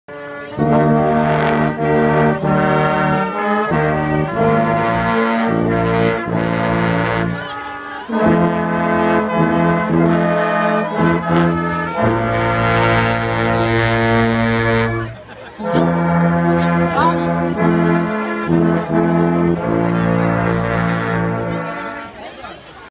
Lake Forest High School Holiday Brass Ensemble
At Lake Bluff's "It's a Wonderful Life!" event Dec 3, 2016